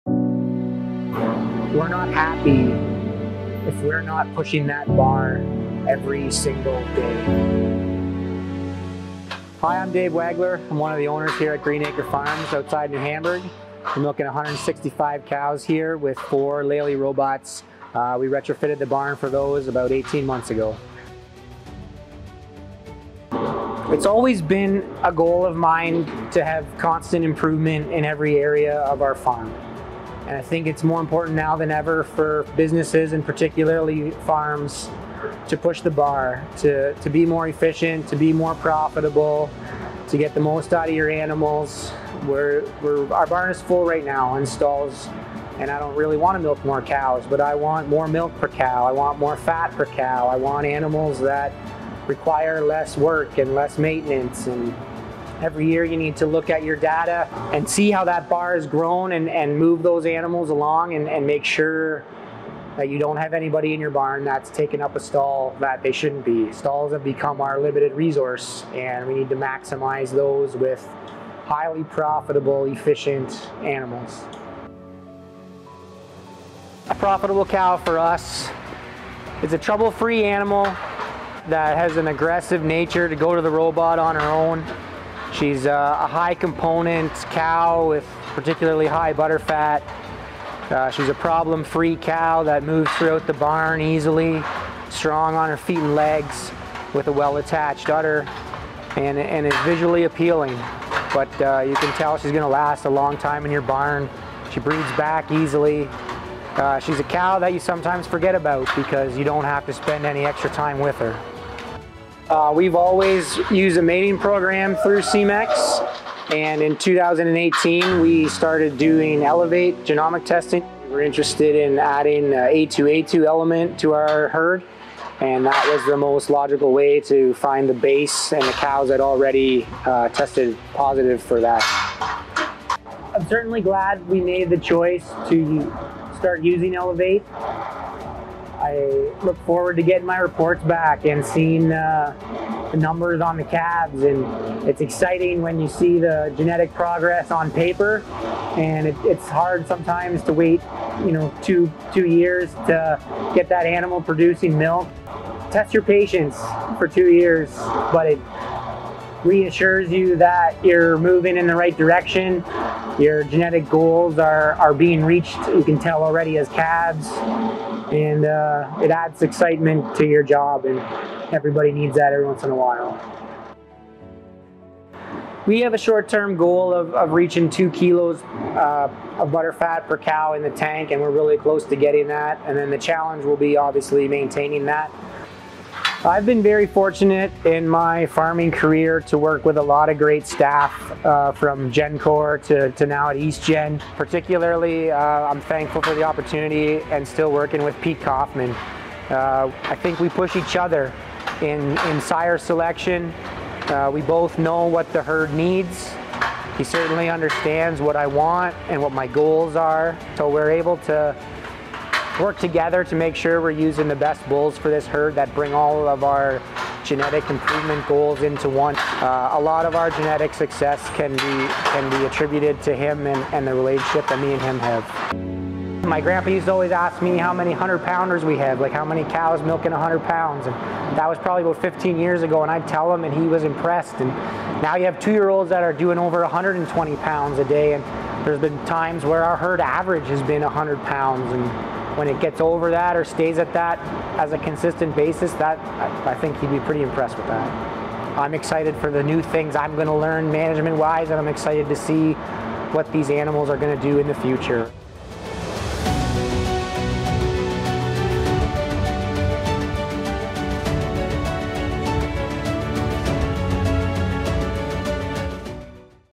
Further Faster: Green Acres Nov 2024
𝗪𝗵𝗮𝘁 𝗔𝗿𝗲 𝗣𝗲𝗼𝗽𝗹𝗲 𝗦𝗮𝘆𝗶𝗻𝗴 𝗔𝗯𝗼𝘂𝘁 𝗚𝗲𝗻𝗼𝗺𝗶𝗰 𝗧𝗲𝘀𝘁𝗶𝗻𝗴 𝗪𝗶𝘁𝗵 𝗘𝗹𝗲𝘃𝗮𝘁𝗲®?
"Stalls are a limited resource; we need to maximize those with highly profitable, efficient animals," says Dave Wagler, Green Acre Farms in Ontario. "In 2018, we started using Elevate testing, and we wanted to add A2A2, so it was the most logical way to find a base. I look forward to getting my reports back and getting the numbers on the calves, and it's exciting to see the genetic progress." Learn more about how genomic testing has changed his 165-cow robot dairy. Working with Semex, he's going further, faster.